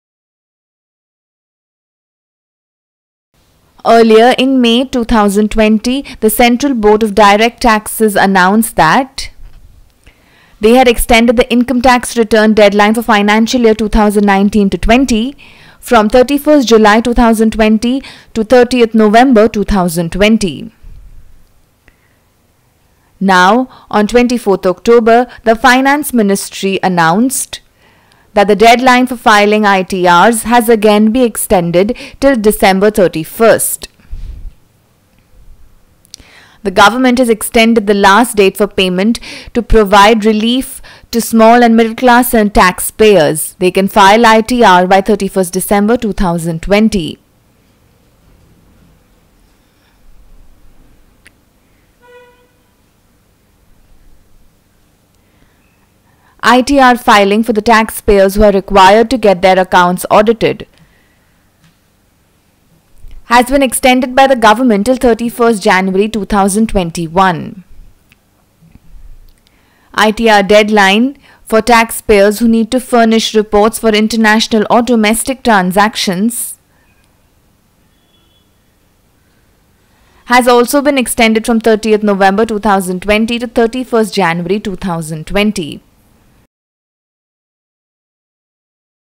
Income Tax Return Deadline Extended
Earlier in May, the Central Board of Direct Taxes (CBDT) had extended the Income Tax Return deadline for FY 2019-20 from 31st July 2020 to 30 November 2020. To provide relief to taxpayers while filing tax returns. Now The Finance Ministry on Saturday announced that the deadline for filing income tax returns (ITRs) has again been extended till December 31. The government has extended the last date for the payment tax to provide relief to small and middle-class taxpayers. ITR filing for the taxpayers who are required to get their accounts audited has been extended by the government by two months till January 31, 2021. The deadline for filing the tax returns for those taxpayers who need to furnish reports concerning international or specific domestic transactions has also been extended from November 30, 2020, to January 31, 2021.